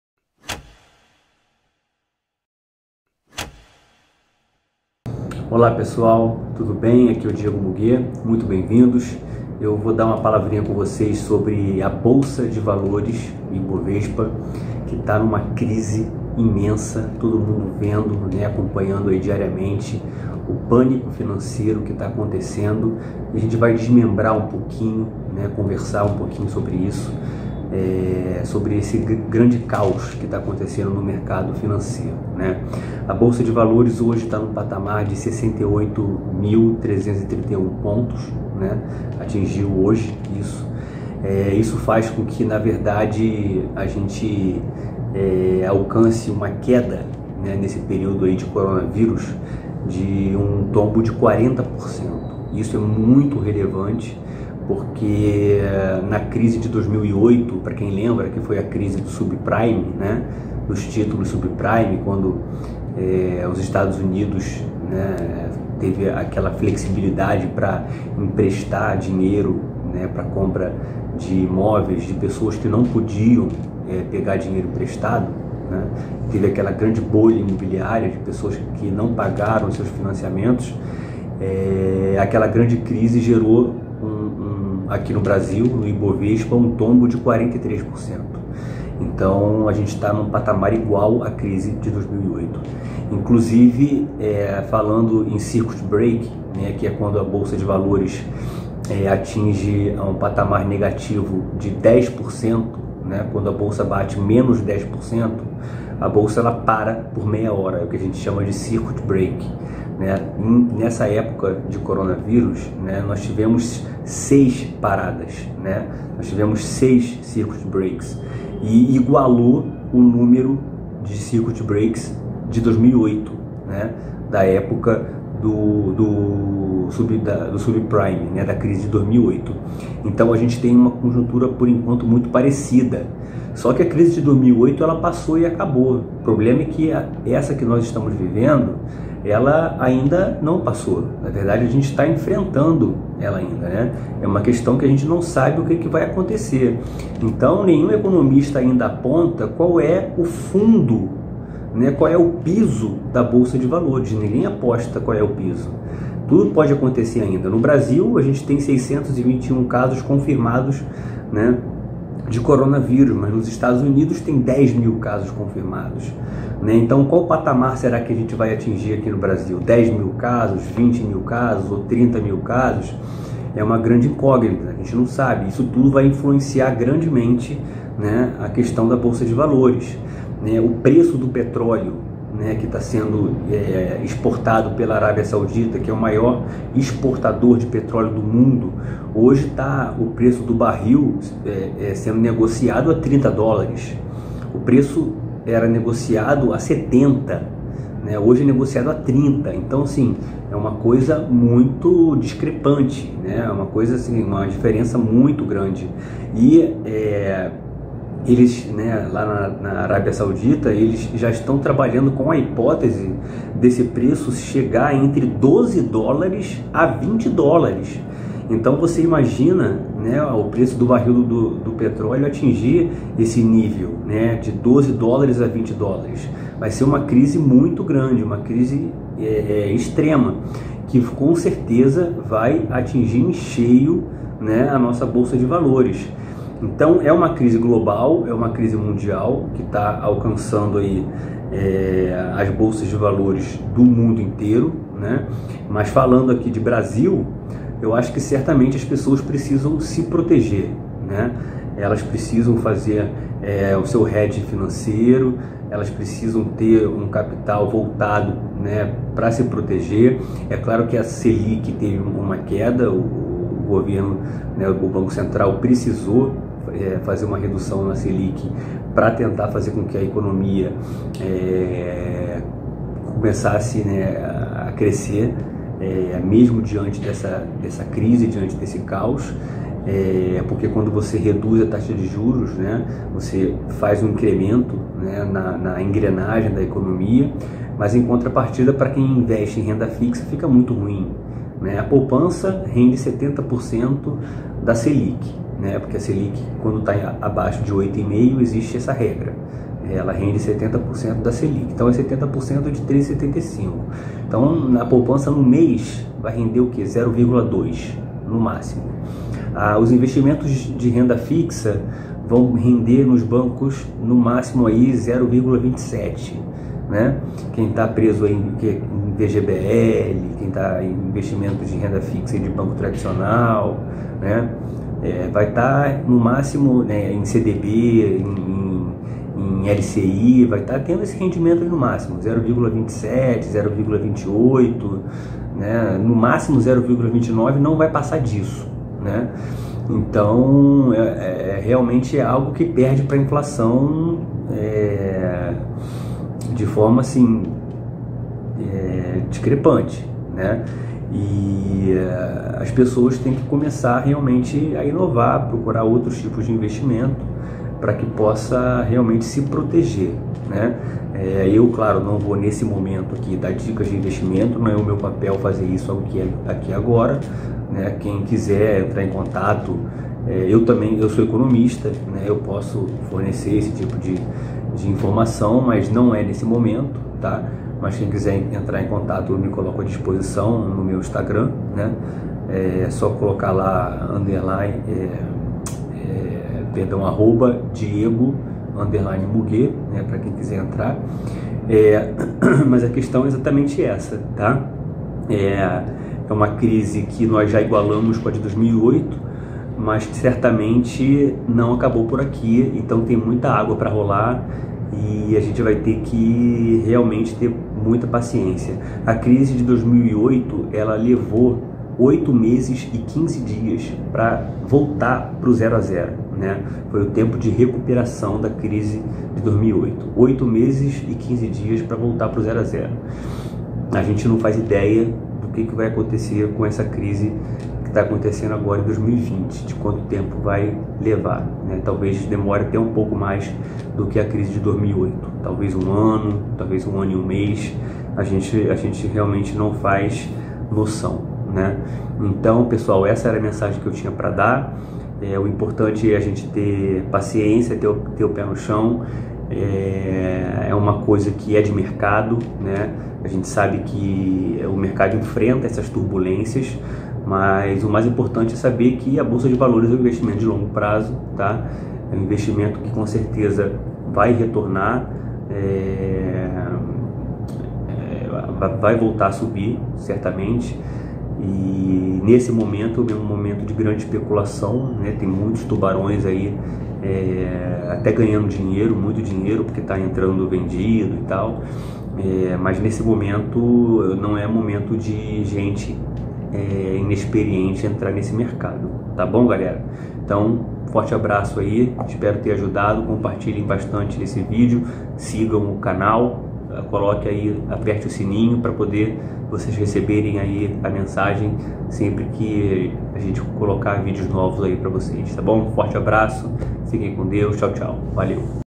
QUEBRA DA BOLSA DE VALORES | Fala Muguet
Como a quebra da Bolsa afeta a sua vida